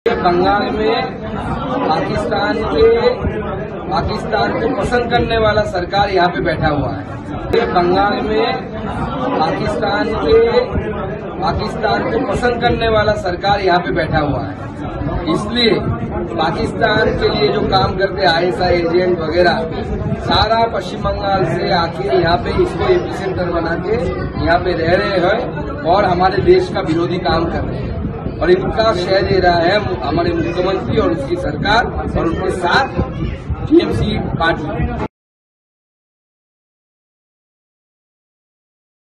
'TMC Govt In Bengal Is Pakistan-Loving Govt': BJP's Sukanta Majumdar
West Bengal BJP president Sukanta Majumdar slammed the ruling TMC government in West Bengal, saying, "The current government in West Bengal is a Pakistan-loving government. This is the reason why people working for Pakistan such as ISI agents are staying in West Bengal and using it as the epicentre to work against our country."